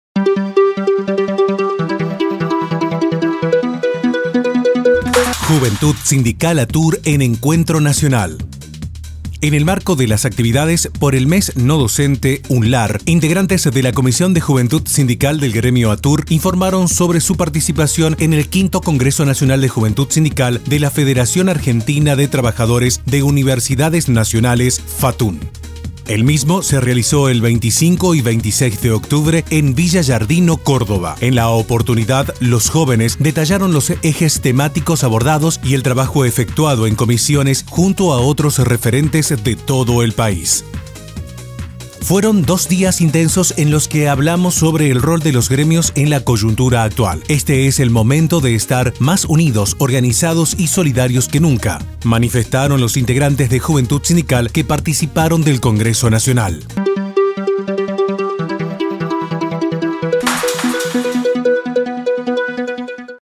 U Noticias - Juventud Sindical ATUR en encuentro nacional
Integrantes de la Comisión de Juventud sindical del gremio ATUR, informaron sobre su participación en el 5° Congreso Nacional de Juventud Sindical de la Federación Argentina de Trabajadores de Universidades Nacionales (FATUN). El mismo se realizó el 25 y 26 de octubre en Villa Giardino, Córdoba.